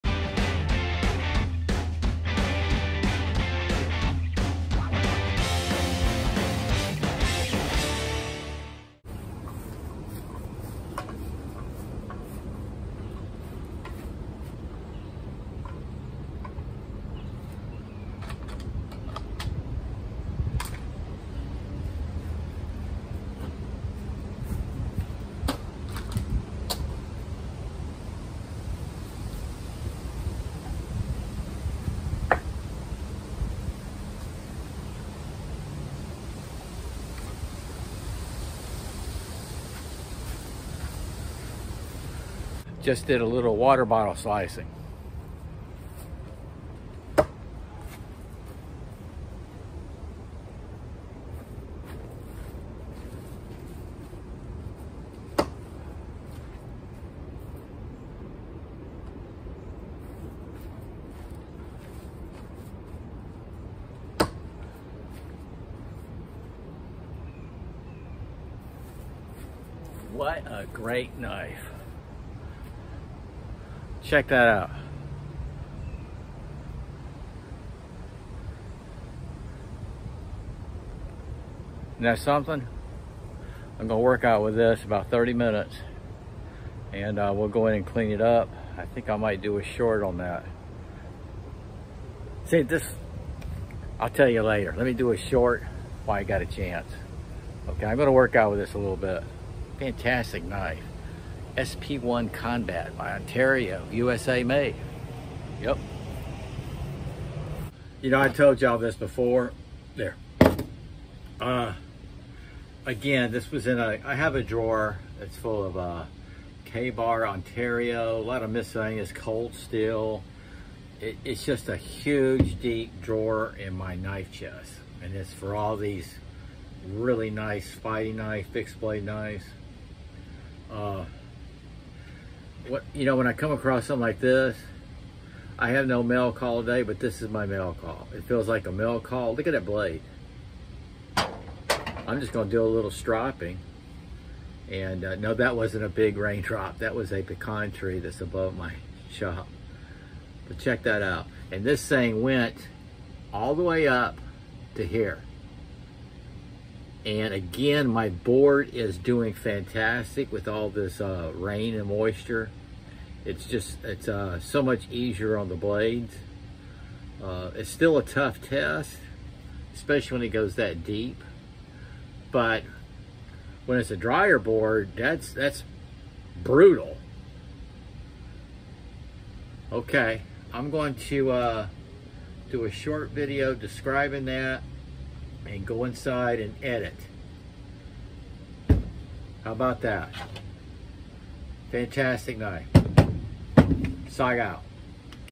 Ontario SP1 Combat Fixed Blade Knife from the Shop to throwing in the Board in the Playground
Sharp and Balanced